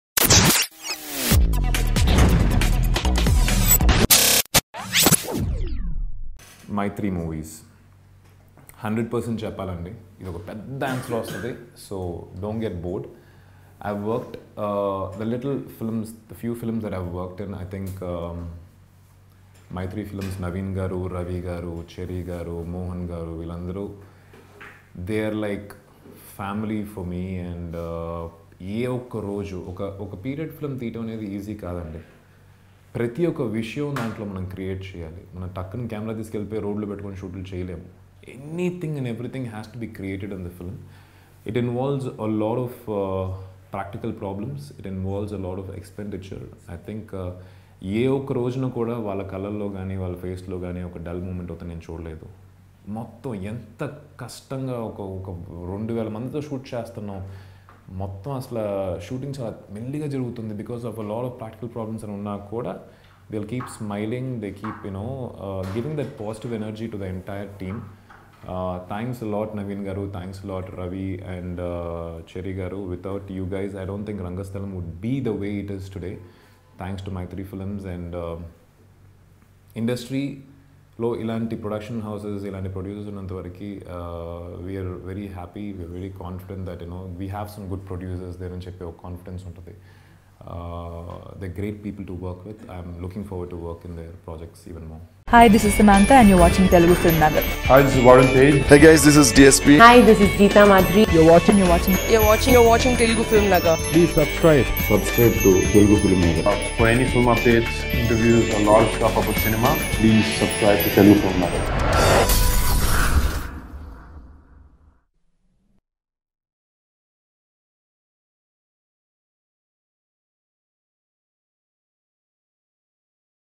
Aadhi Pinisetty Heartfelt Words about Rangasthalam Producers | Interview | Ram Charan | Samantha
Aadhi Pinisetty Heartfelt Words about Rangasthalam Producers in a latest Rangasthalam movie interview on Telugu FilmNagar. Rangasthalam 2018 latest Telugu movie ft. Ram Charan, Samantha, Pooja Hegde and Aadhi Pinisetty. Directed by Sukumar. Music by DSP / Devi Sri Prasad. Produced by Mythri Movie Makers.